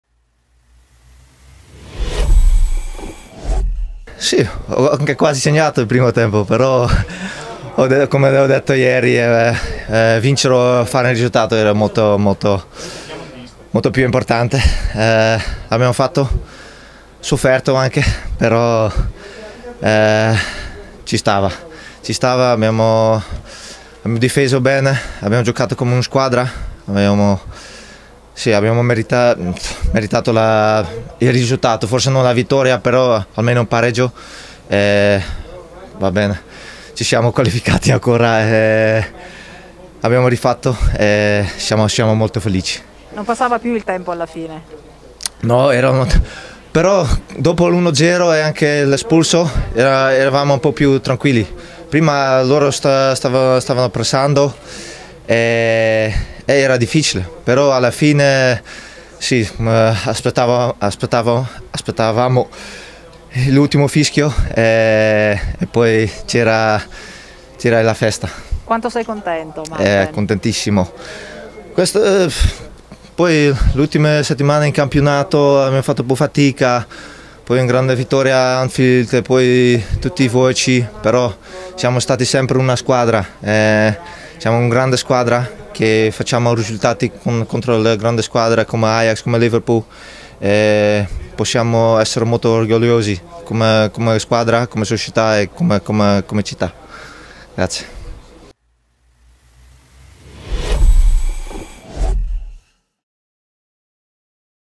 UCL MD6 | Ajax-Atalanta | Marten de Roon: "Abbiamo giocato da squadra"
Tutta la soddisfazione di Marten deRoon che finalmente vince alla Johan Cruijff Arena di Amsterdam, non ha segnato, ma va bene lo stesso come il centrocampista nerazzurro afferma aggiungendo: "Abbiamo giocato da squadra".

All the satisfaction of Marten de Roon who finally wins at the Johan Cruijff Arena in Amsterdam, he did not score, but it's okay as the Nerazzurri midfielder says adding: "We played as a team".